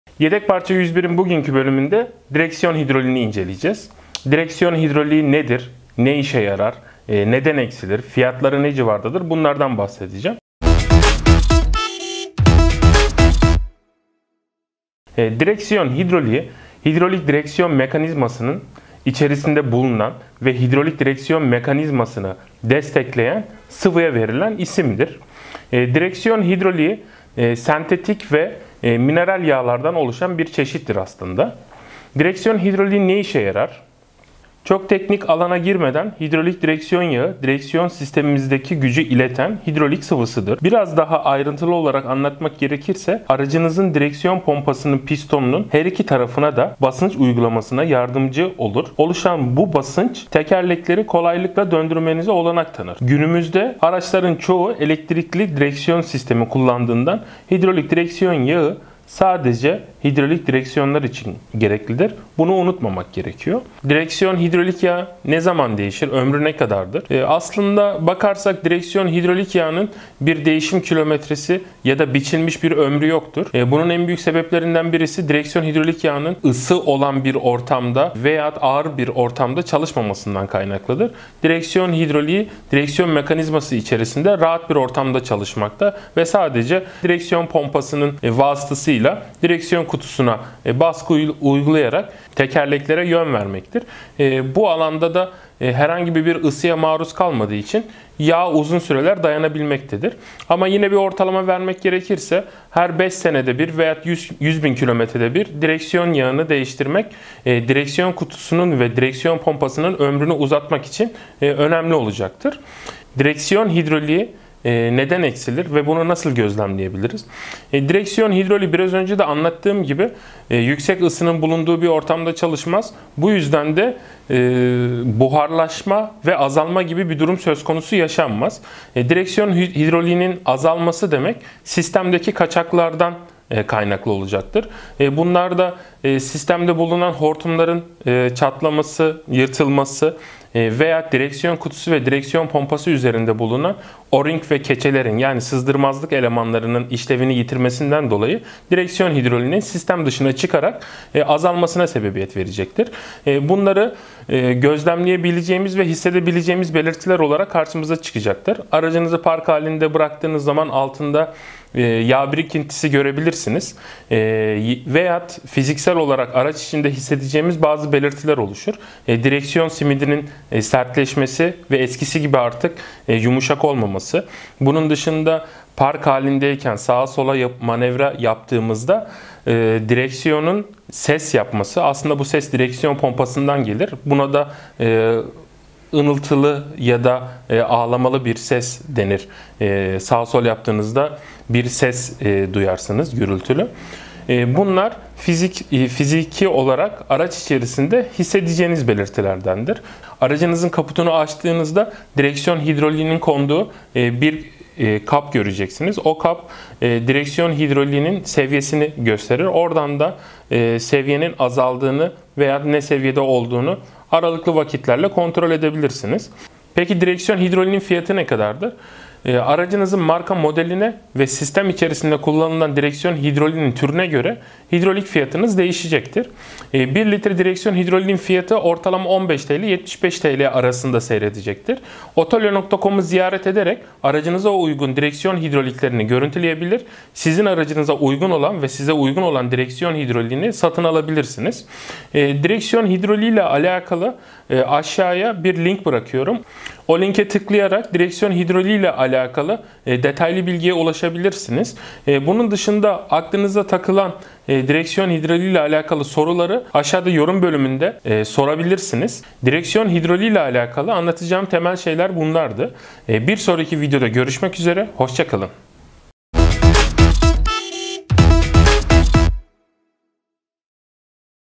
Direksiyon Hidrolik Yağı Nedir, Nasıl Değiştirilir ? - Yedek Parça 101
Direksiyon hidrolik yağı bir otomobilin direksiyon sisteminin en önemli bileşenlerinden biri olmasına rağmen, direksiyon hidrolik yağı genellikle ihmal edilir ve sürücülerin, araçlarının direksiyon hidroliğini nasıl kontrol edeceğinden haberleri dahi yoktur. Temel olarak direksiyon hidroliğinin ne olduğu, nasıl çalıştığı ve nasıl bakım yapılması gerektiği hakkında bilgi sahibi olarak, aracınızın direksiyon sisteminin her daim yüksek performansta çalıştığından emin olabilirsiniz. Bu yayınımızda direksiyon hidroliği ile alakalı kafanızda soru işareti bırakmayacak eksiksiz bir rehber sunuyoruz.

Direksiyon hidrolik sıvısı, hidrolik direksiyon sistemi için hidrolik destek sağlar. Direksiyon hidrolik yağı, köpürmeyi önlemek, korozyonu bastırmak ve hidrolik direksiyon pompası ve direksiyon dişlisinde yüksek performansta yağlama yapmak için katkılarla güçlendirilmiş mineral yağ veya sentetik yağlardır.